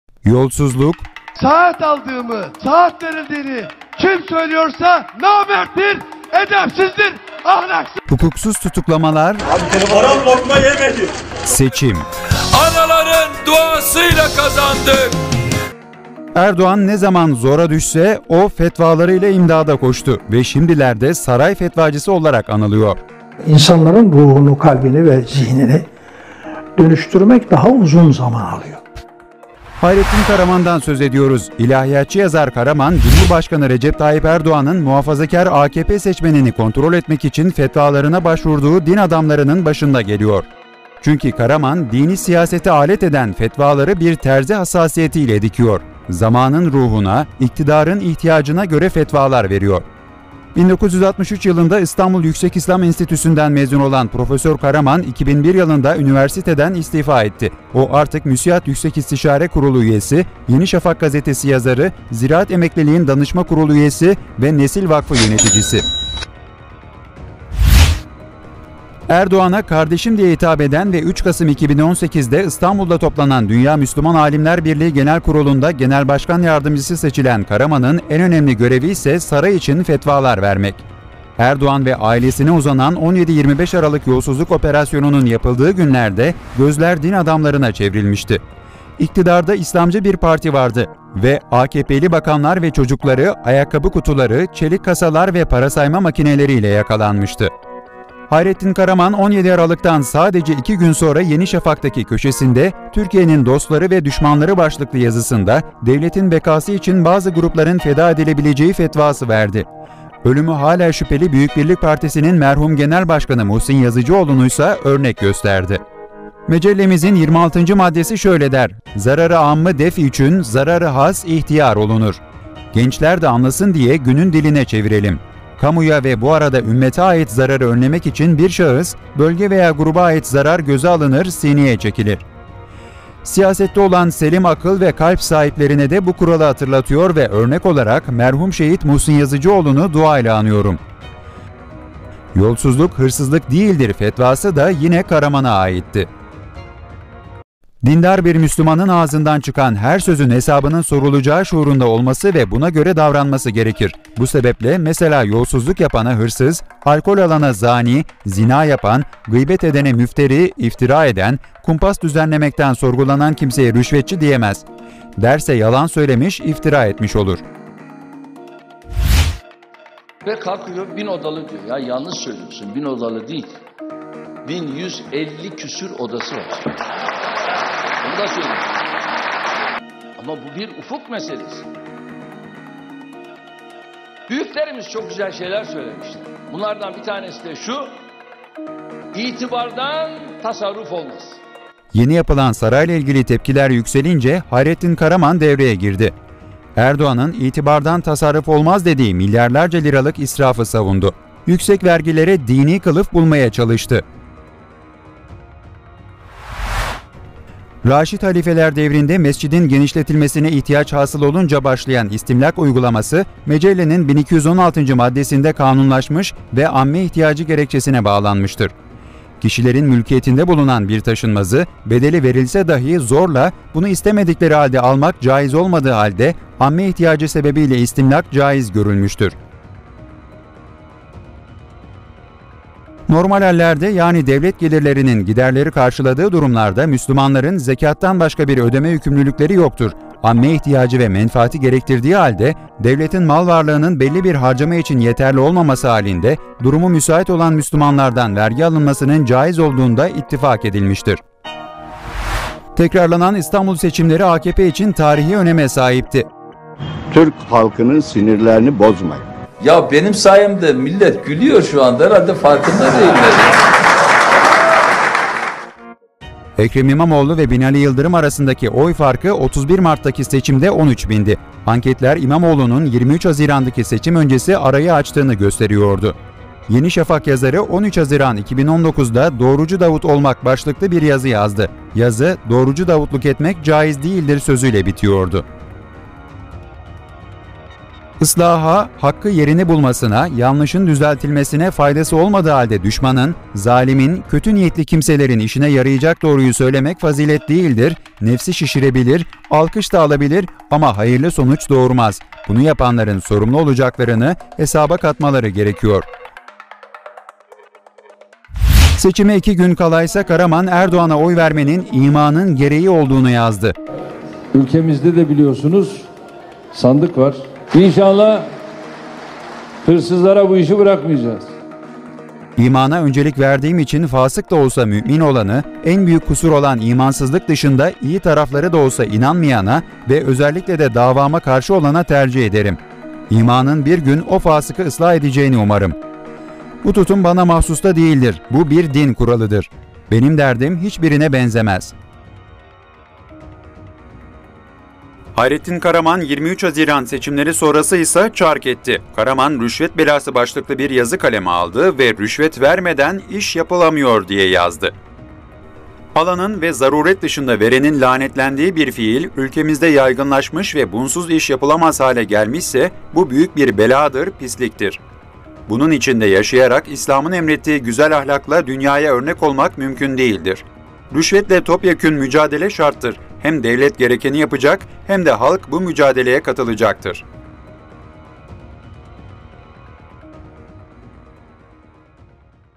Saray fetvacısı Hayrettin Karaman'ın rüşvet çarkı
Yolsuzluk, hukuksuz tutuklamalar, seçim.

Erdoğan ne zaman zora düşse o, fetvalarıyla imdada koştu.

Ve şimdilerde 'Saray fetvacısı' olarak anılıyor.

Hayrettin Karaman'dan söz ediyoruz.

İlahiyatçı yazar Karaman, Cumhurbaşkanı Recep Tayyip Erdoğan'ın muhafazakar AKP seçmenini kontrol etmek için fetvalarına başvurduğu din adamlarının başında geliyor.

Çünkü Karaman, dini siyasete alet eden fetvaları bir terzi hassasiyetiyle dikiyor.

Zamanın ruhuna, iktidarın ihtiyacına göre fetvalar veriyor.

1963 yılında İstanbul Yüksek İslâm Enstitüsü'nden mezun olan Profesör Karaman, 2001 yılında üniversiteden istifa etti.

O artık MÜSİAD Yüksek İstişare Kurulu üyesi, Yenişafak Gazetesi yazarı, Ziraat Emeklilik’in Danışma Kurulu üyesi ve Nesil Vakfı yöneticisi.

Erdoğan'a 'kardeşim' diye hitap eden ve 3 Kasım 2018 de İstanbul'da toplanan Dünya Müslüman Alimler Birliği Genel Kurulu'nda Genel Başkan yardımcısı seçilen Karaman'ın en önemli görevi ise Saray için fetvalar vermek.

Erdoğan ve ailesine uzanan 17-25 Aralık yolsuzluk operasyonun yapıldığı günlerde gözler din adamlarına çevrilmişti.

İktidarda İslamcı bir parti vardı.

Ve AKP'li bakanlar ve çocukları ayakkabı kutuları, çelik kasalar ve para sayma makineleri ile yakalanmıştı.

Hayrettin Karaman, 17 Aralık'tan sadece 2 gün sonra Yeni Şafak'taki köşesinde 'Türkiye'nin dostları ve düşmanları' başlıklı yazısında devletin bekası için bazı grupların feda edilebileceği fetvası verdi.

Ölümü hala şüpheli Büyük Birlik Partisi'nin merhum genel başkanı Muhsin Yazıcıoğlu'nu ise örnek gösterdi.

GRAFİK

HAYRETTİN KARAMAN/ YENİ ŞAFAK YAZARI

Mecellemizin 26. Maddesi şöyle der: "Zarar-ı âmmı def"içün zarar-ı hâss ihtiyar olunur".

Gençler de anlasın diye günün diline çevirelim:

Kamuya (ve bu arada ümmete) ait zararı önlemek için bir şahıs, bölge veya gruba ait zarar göze alınır, sineye çekilir.

Siyasette olan selim akıl ve kalb sahiplerine de bu kuralı hatırlatıyor ve örnek olarak merhum şehid Muhsin Yazıcıoğlu"nu dua ile anıyorum.

'Yolsuzluk hırsızlık değildir' fetvası da yine Karaman'a aitti.

GRAFİK

... dindar bir Müslümanın ağzından çıkan her sözün hesabının sorulacağı şuurunda olması ve buna göre davranması gerekir. Bu sebeple mesela yolsuzluk yapana “hırsız”, alkol alana “zânî: zina yapan”, gıybet edene “müfteri: iftira eden”, “kumpas düzenlemekten sorgulanan kimseye rüşvetçi” diyemez. Derse yalan söylemiş, iftira etmiş olur.

Türkiye'nin milyarlarca lira harcanan Cumhurbaşkanlığı Sarayı için yapılan harcamaları eleştirdiği günlerde Hayrettin Karaman 'Erdoğan'ın itibardan tasarruf olmaz'

Yeni yapılan Saray'la ilgili tepkiler yükselince Hayrettin Karaman devreye girdi.

Tekrarlanan İstanbul seçimleri AKP için tarihi öneme sahipti.

Ekrem İmamoğlu ve Binali Yıldırım arasındaki oy farkı 31 Mart'taki seçimde 13 bindi.

Anketler İmamoğlu'nun 23 Haziran'daki seçim öncesi arayı açtığını gösteriyordu.

Yenişafak yazarı 13 Haziran 2019'da "Doğrucu Davud olmak" başlıklı bir yazı yazdı.

Yazı, "Doğrucu Davutluk etmek caiz değildir." sözüyle bitiyordu.

Seçime 2 gün kala ise Karaman, Erdoğan'a oy vermenin imanın gereği olduğunu yazdı.

HAYRETTİN KARAMAN :

"İmana öncelik verdiğim için fâsık da olsa mümin olanı, en büyük kusur olan imansızlık dışında iyi tarafları da olsa inanmayana ve özelikle de davama karşı olana tercih ederim. İmanın bir gün o fâsıkı ıslah edeceğini umarım. Bu tutum bana mahsus da değildir, bu bir din kuralıdır. “Benim derdim hiçbirine benzemez- 21 Haziran 2019"

Hayrettin Karaman, 23 Haziran seçimleri sonrası ise çark etti.
Karaman 'Rüşvet Belası' başlıklı bir yazı kaleme aldı. Ve “Rüşvet vermeden iş yapılamıyor." diye yazdı.
GRAFİK
Hayrettin Karaman / 01 Ağustos 2019 Rüşvet Belası
"Alanın ve -zaruret dışında- verenin lanetlendiği bir fiil ülkemizde yaygınlaşmış ve bunsuz iş yapılamaz hale gelmiş ise bu büyük bir belâdır, pisliktir, bunun içinde yaşayarak İslam’ın emrettiği güzel ahlak ile dünyaya örmek olmak mümkün değildir. Rüşvet ile topyekun mücadele şarttır; hem devlet gerekeni yapacak, hem de halk bu mücadeleye katılacaktır.”